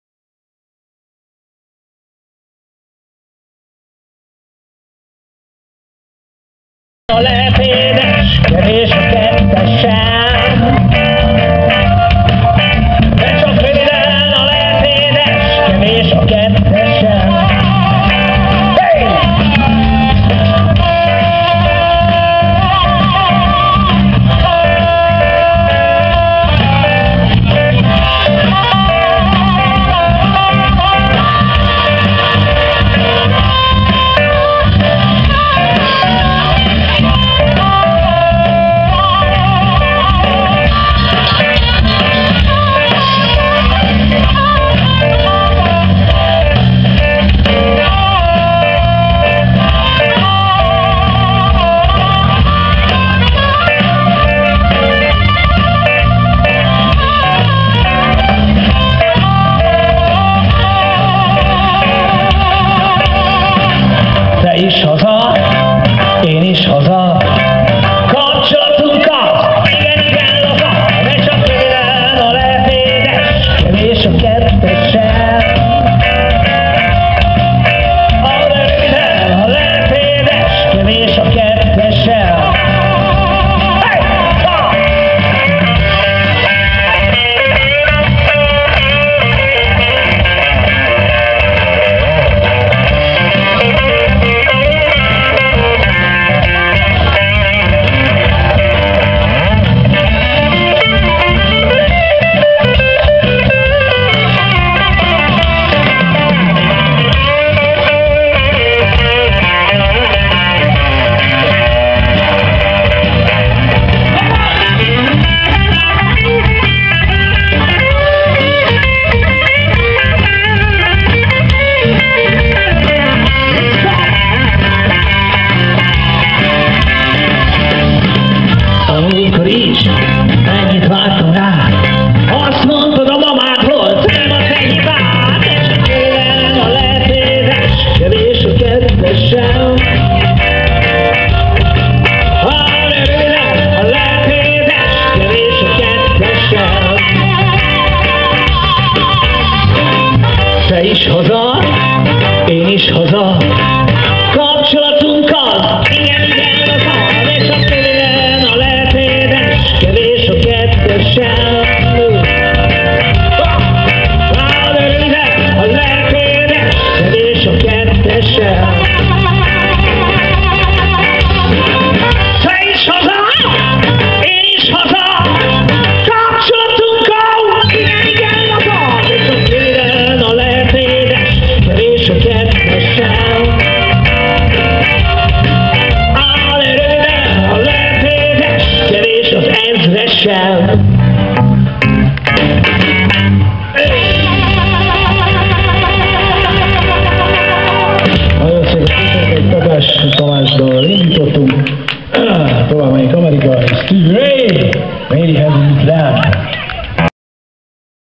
Some Blues
Some Blues 2014 július Békéscsaba Ének: Anthony Szatyi Szathmári, Harp: Jankó László, Bass gitár: Szabó Sándor, Szólógitár: Kovács Pál, Dob: Domokos Doma Attila